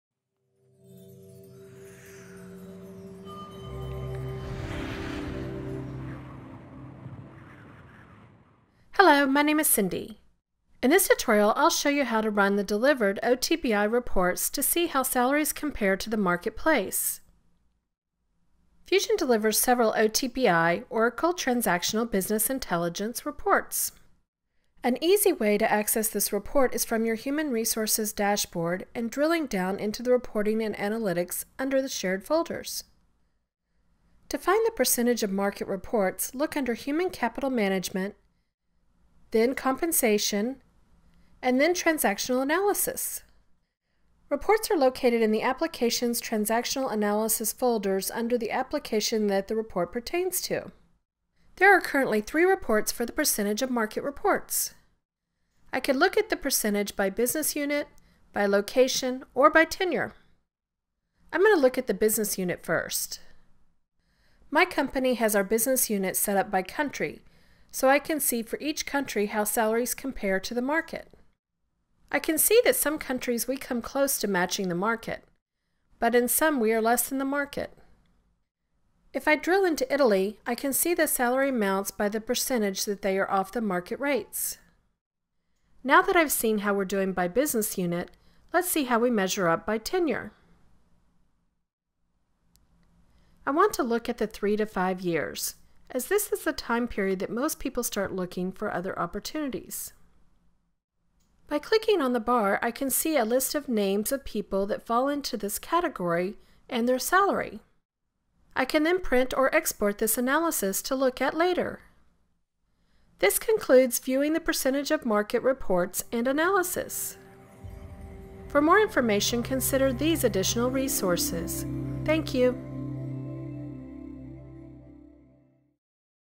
Compensation | Evaluating Salary % of Market with Delivered Analytics
This tutorial applies to Rel 9. It shows you how to run the Percent of Market reports and how this report can be exported.